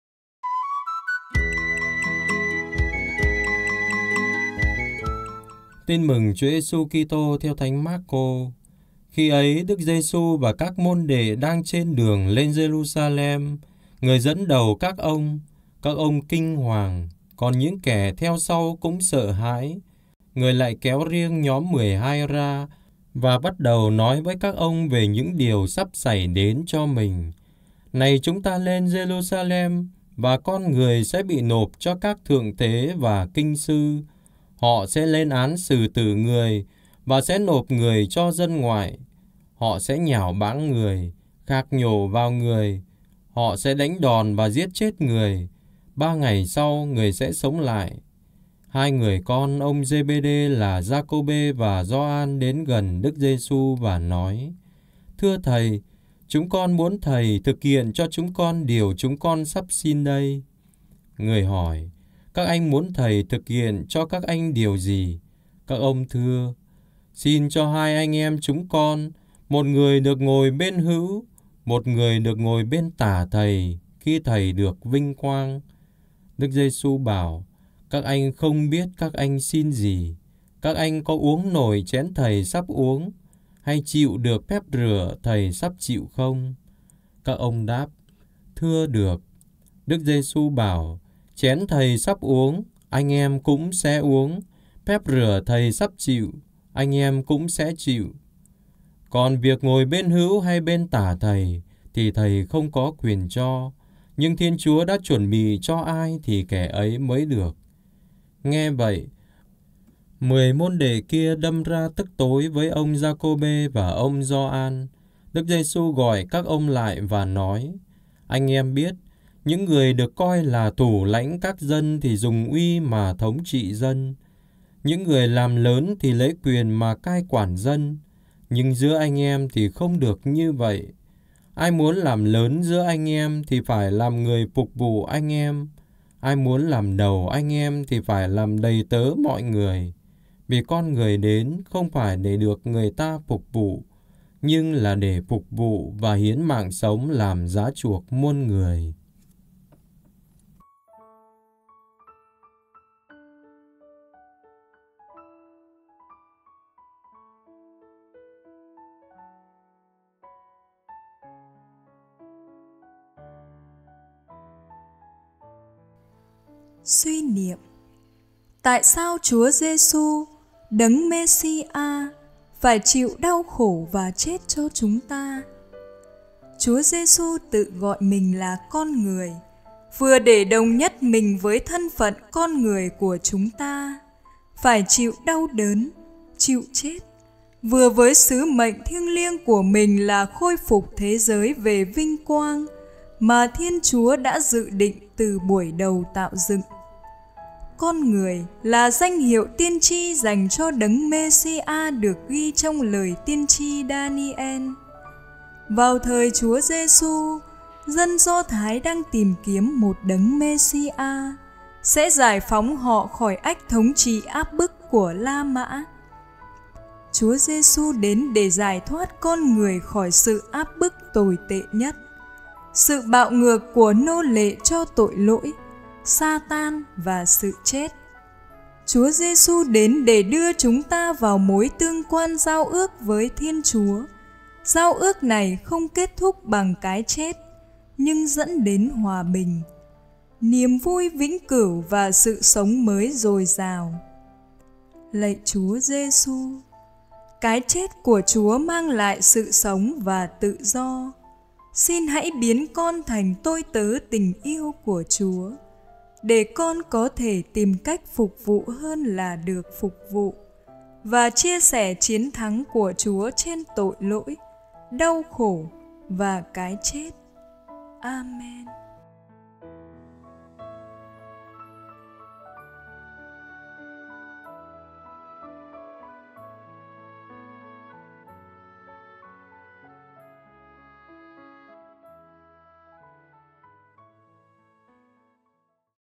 Chúa Giêsu đến để cứu độ chúng ta | Thứ Tư tuần VIII Thường Niên | Lời Chúa hằng ngày
Tin Mừng và suy niệm Tin Mừng | Thứ Tư tuần VIII Thường Niên | Lời Chúa hằng ngày

- Chủ đề: Chúa Giêsu đến để cứu độ chúng ta

Bản quyền nội dung thuộc về Giáo phận Bắc Ninh. Xin không đăng tải lại ở các kênh khác.